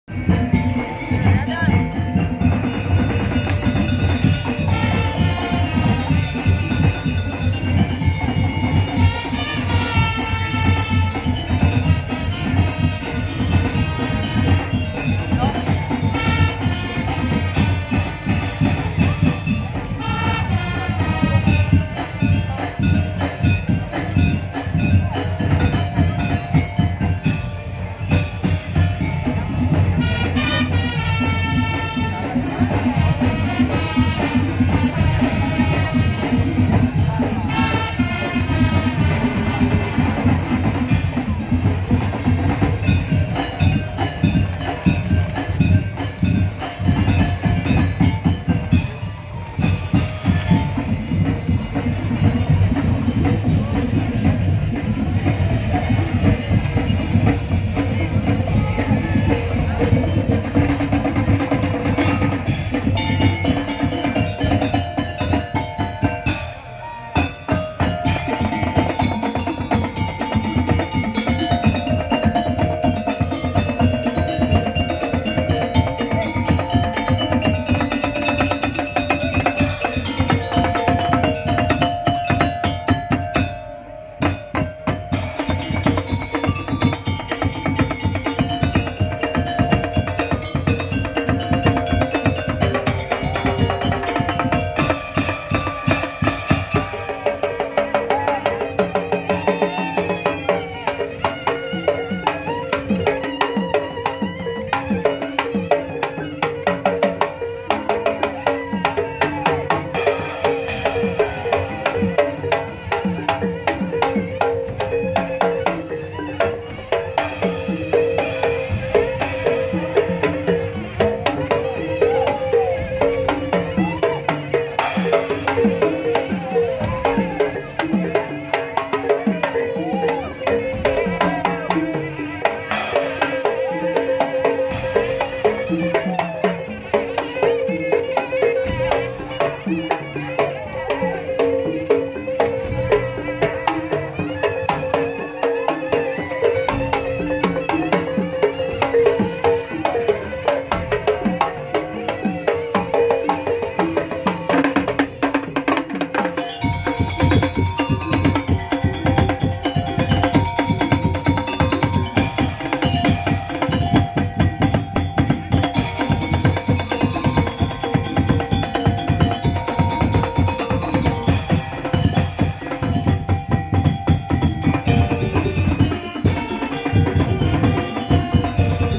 Singo Ireng| Music Tong Tong | Acara Hari Jadi Sumenep Tahun 2023
Video Ini Merupakan Acara Festival Tahunan Di Kapubaten Sumenep Tahun 2023